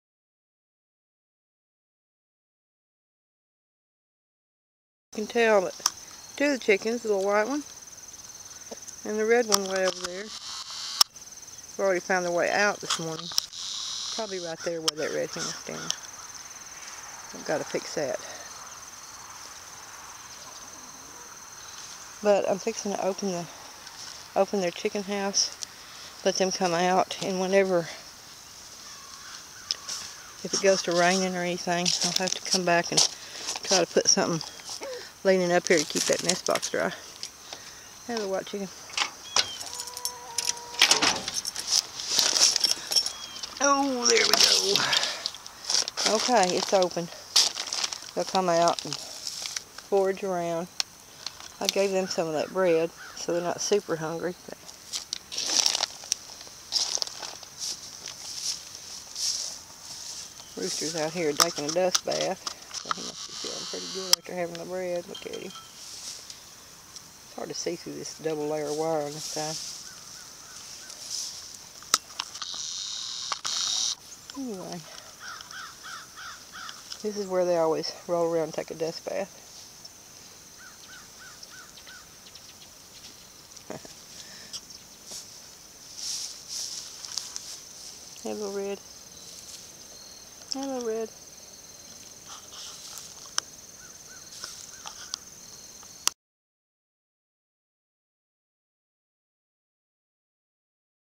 Dust bath chickens
Short video showing my rooster enjoying himself in the dust. No rain for several weeks hurts a lot of things, but it keeps the chickens happy!